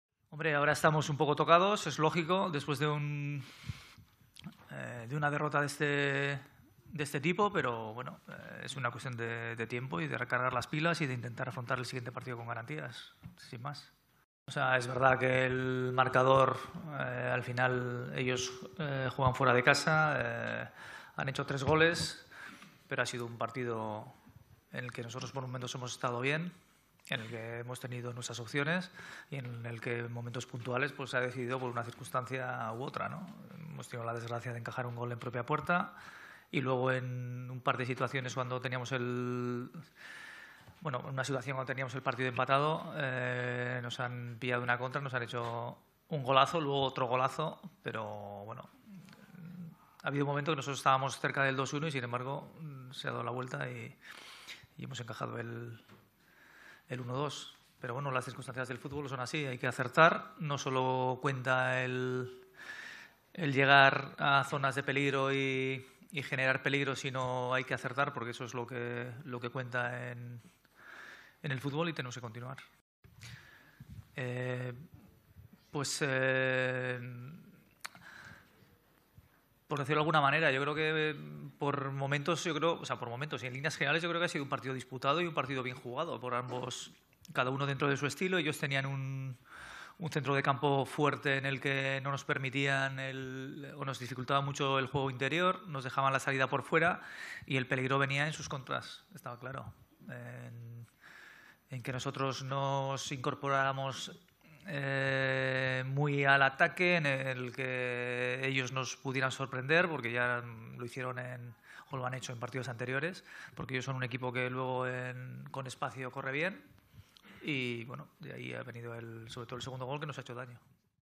Valverde: "Estamos un poco tocados" | Deportes
Ernesto Valverde, entrenador del Barcelona, reconoce que su equipo queda tocado tras la derrota por 1-3 contra el Real Madrid en el Clásico de ida de la Súpercopa de España.